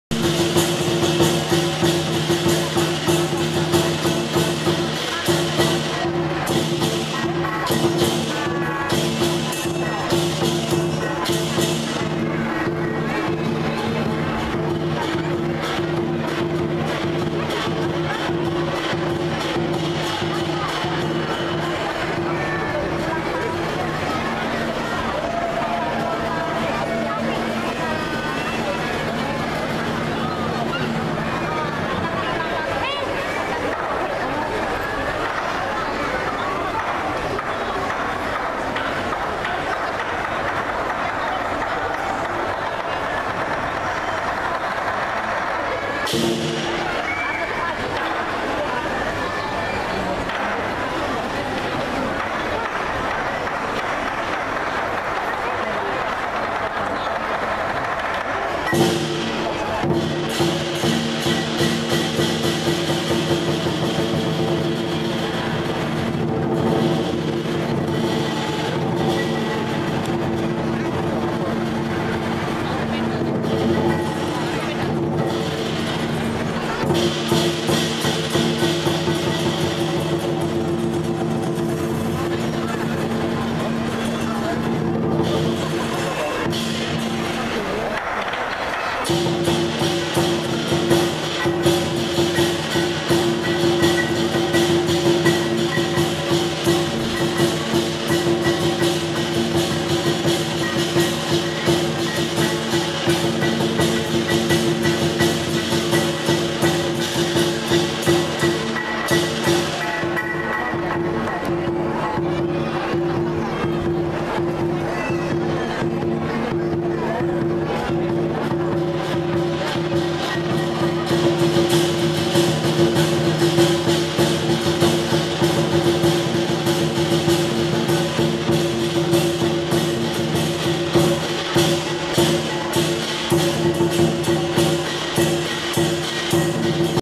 Atraksi Tarian Barongsai Imlek 2017
IMLEK 2017 Lion and Liong Dance Attraction
 Atraksi Barongsai dan Naga Selatan 18 Meter Menyala
 Mall Malioboro Jogja
26 Januari 2017